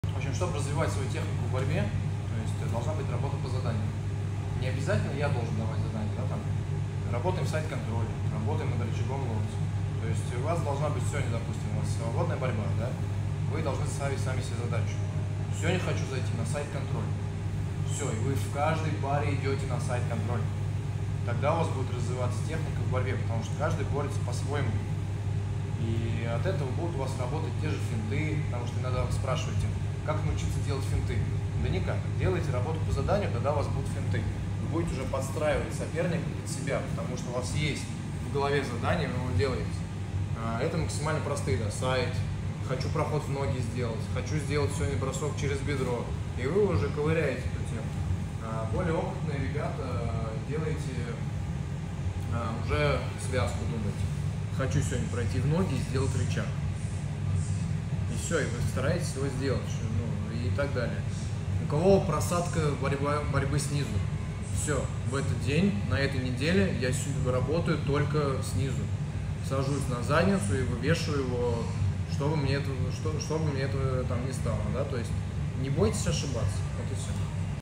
Мысли о работе по заданию в борьбе
Работа по заданию дает развитие вашей борьбы🤙🏻
Давайте себе задание на неделю, на день, на месяц
Не бойтесь совершать ошибки